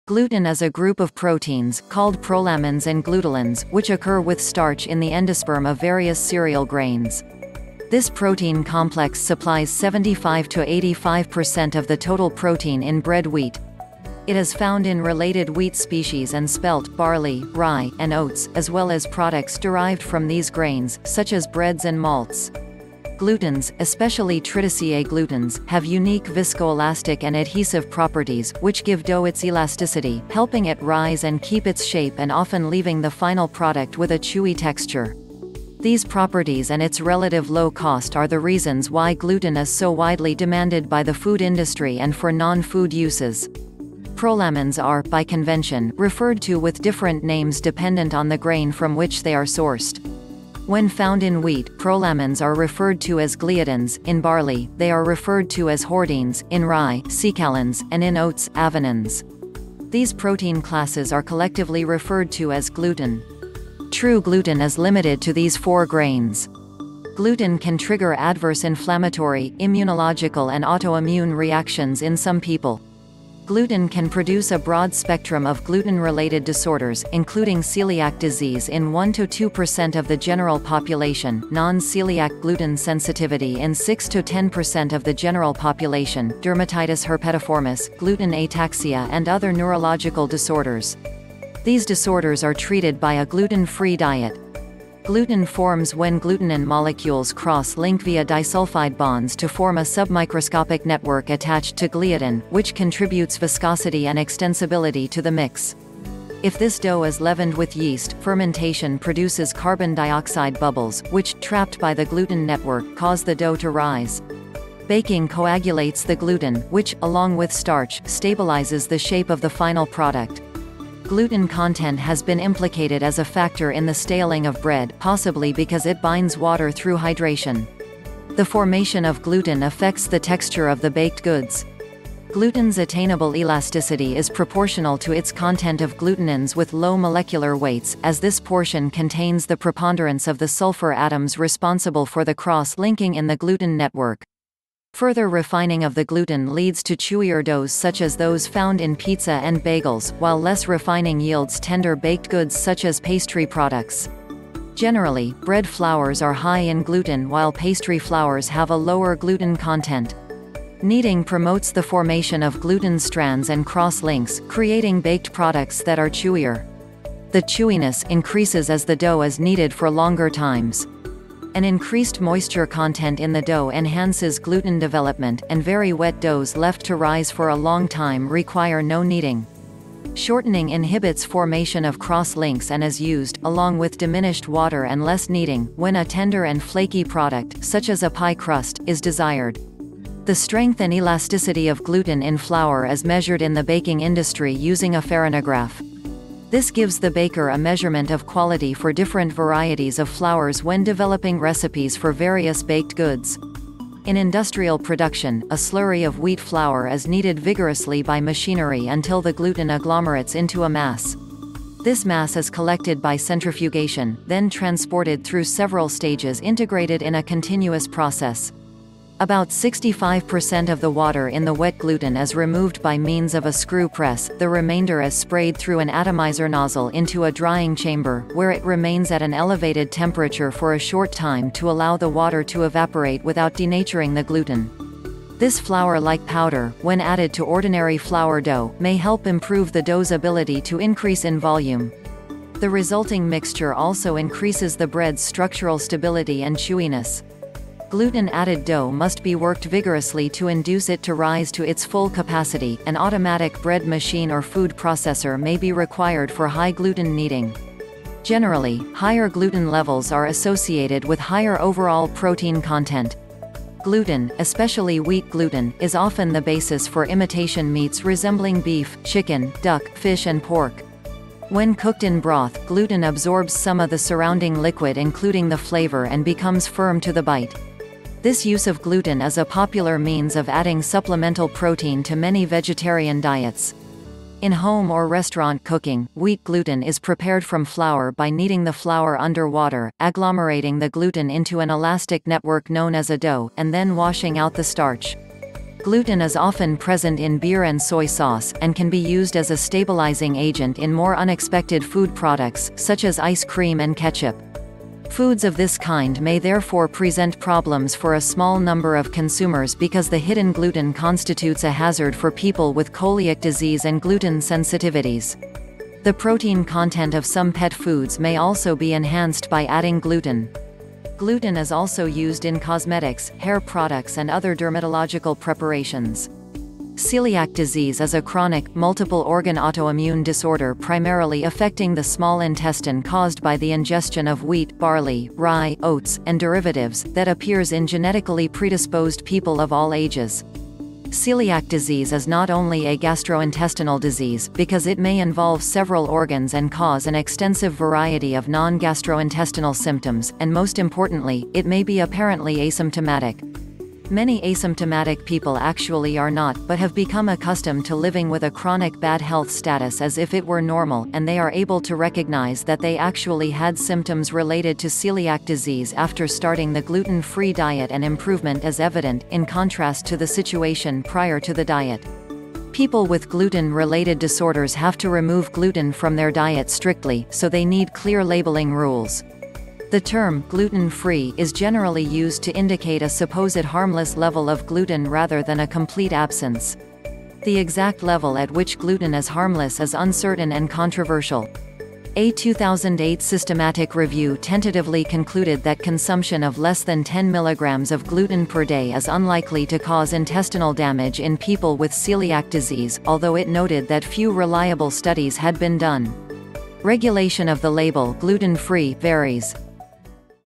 What is gluten and where is it found
Gluten is a group of proteins, called prolamins and glutelins, which occur with starch in the endosperm of various cereal grains. This protein complex supplies 75–85% of the total protein in bread wheat.